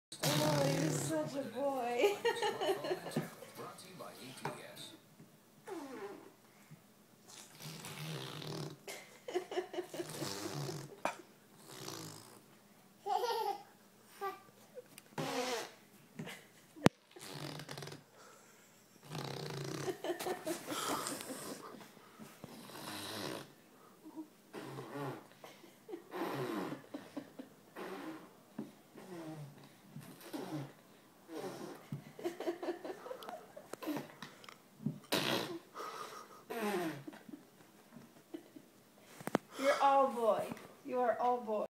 Griffin the Entertainer
Making noises on the end table.